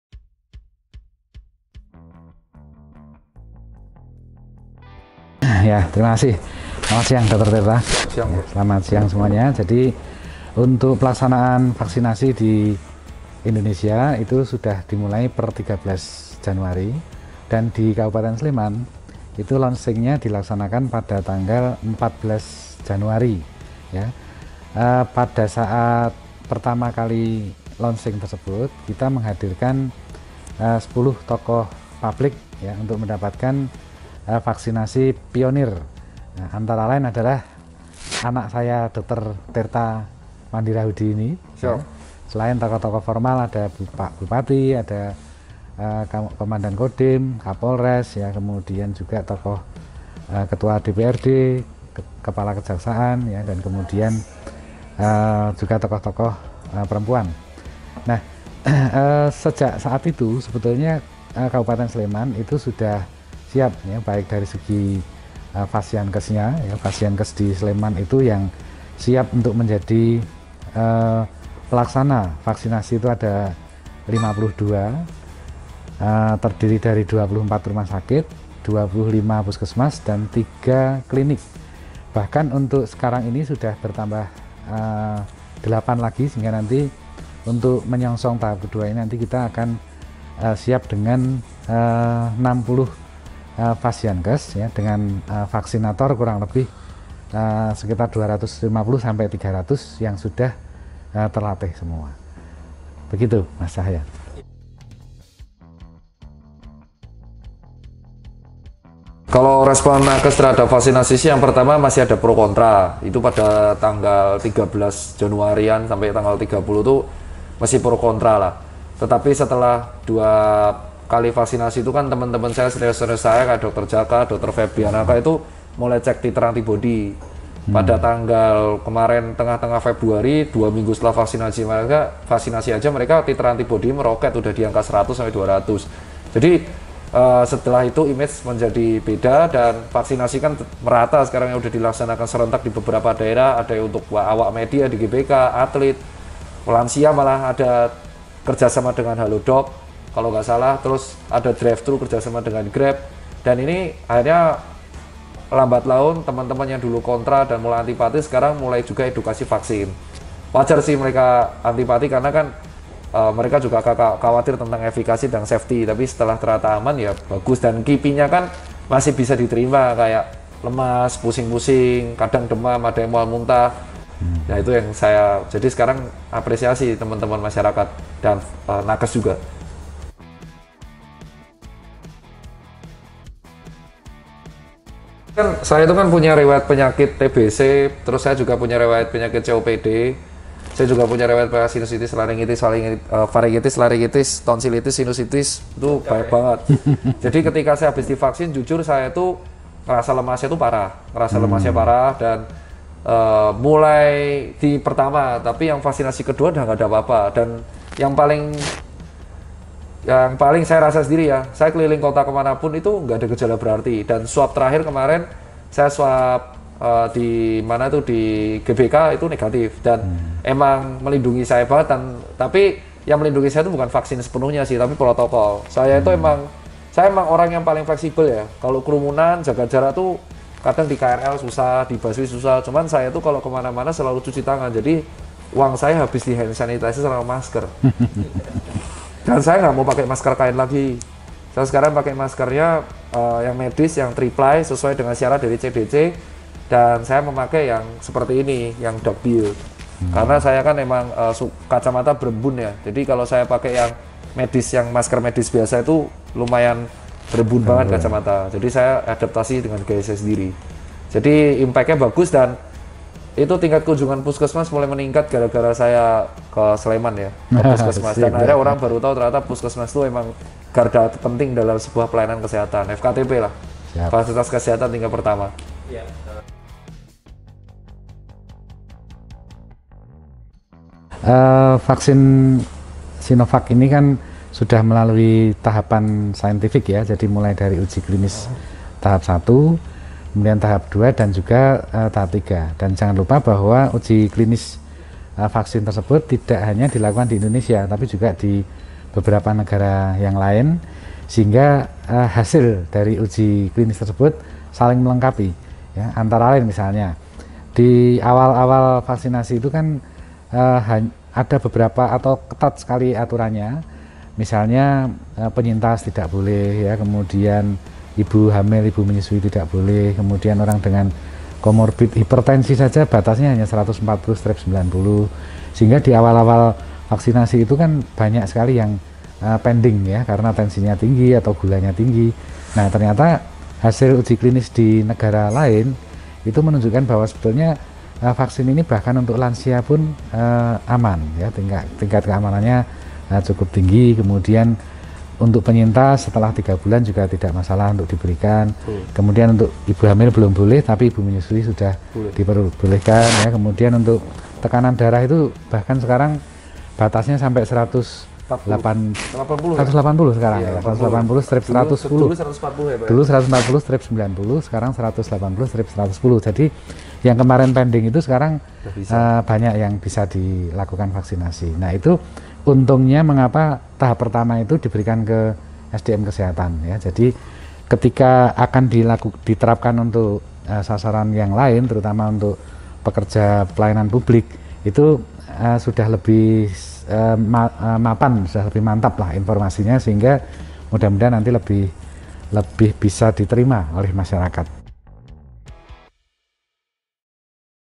Dialog Sehat bagian 1
Kenali kesiapan Pemkab Sleman dalam Program Vaksinasi COVID-19, efektivitas, dan keamanannya dari dr Joko Hastaryo, M.Kes. (Kadinkes Sleman) serta pengalaman edukasi pencegahan hingga paska vaksinasi COVID-19 dari dr. Tirta selaku dokter relawan.